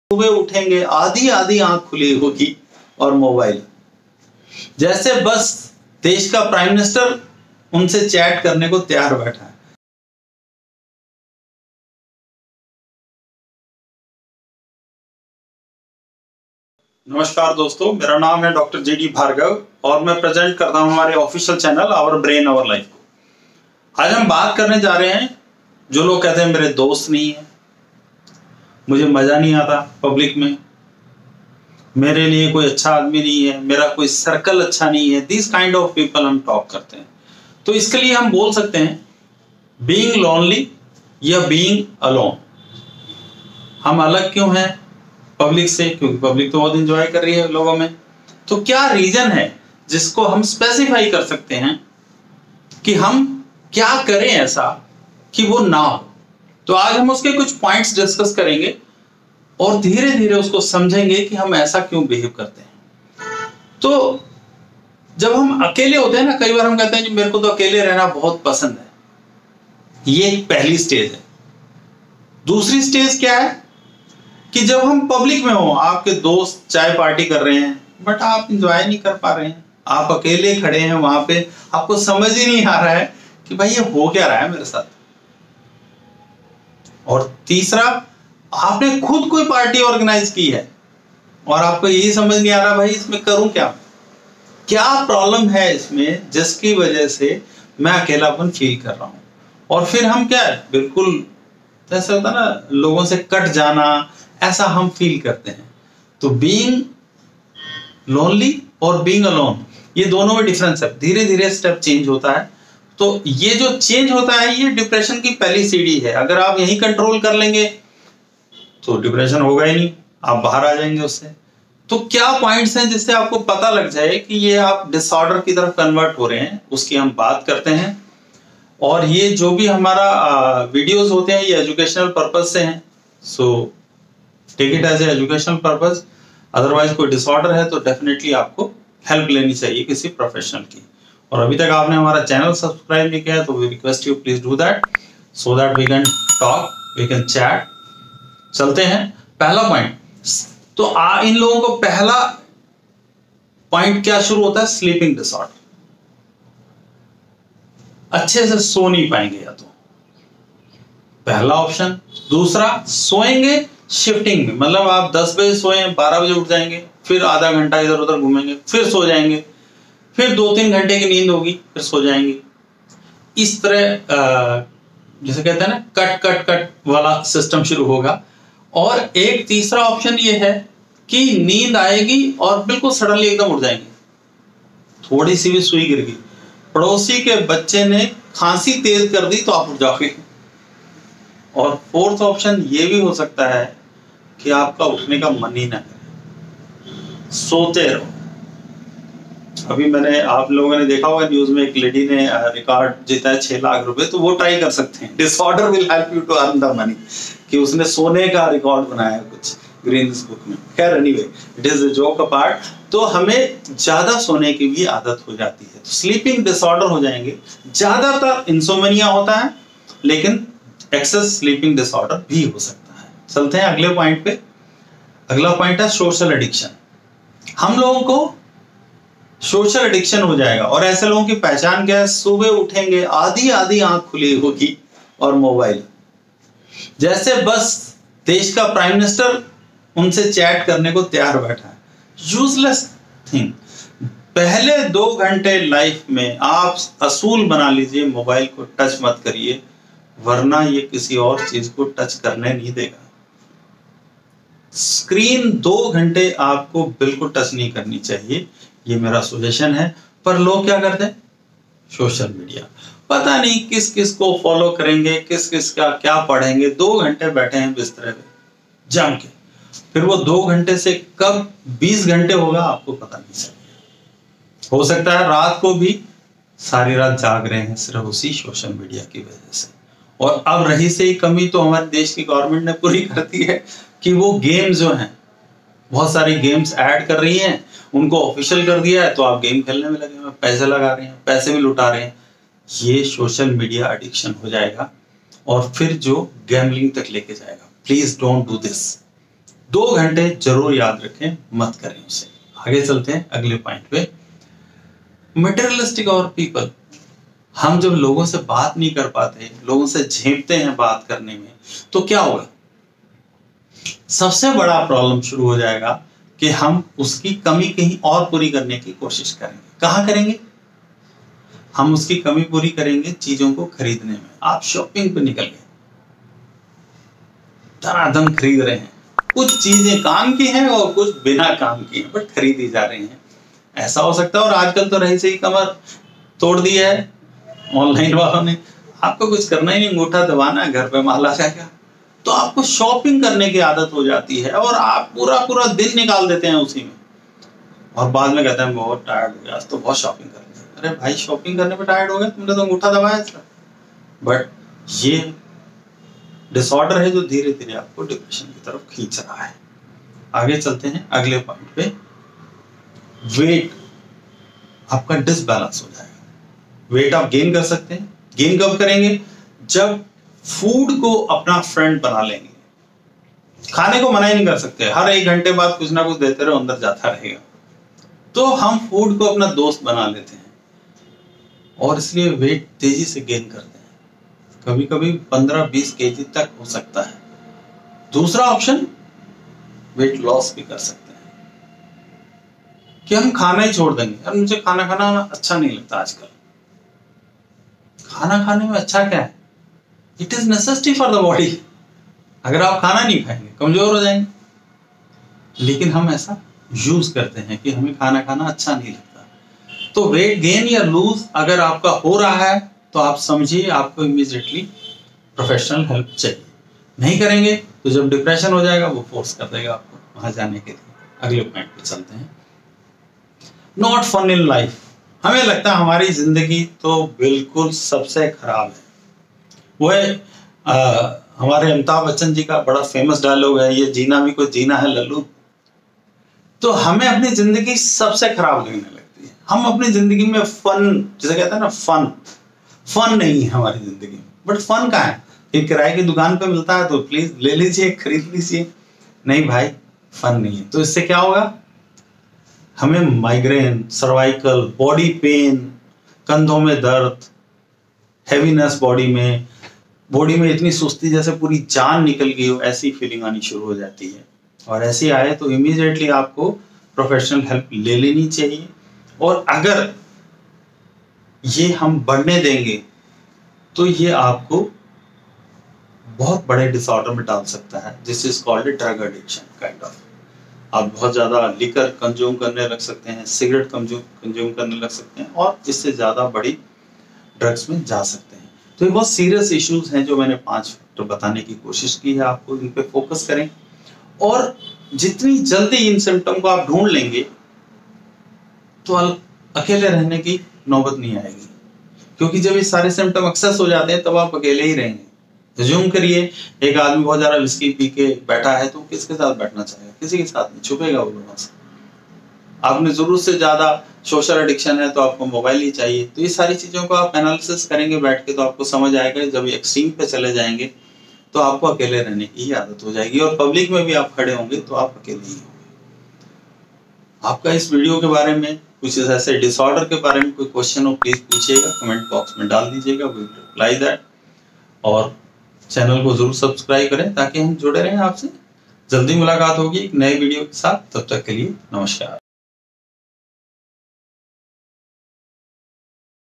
Five points to understand why you stay away from your friends? Being Lonely or Being Alone.
Counseling was not easy . we make it easy
Counselling, Psychology & Wellness center
Our Brain Our Life

Video By Dr.G.D.Bhargava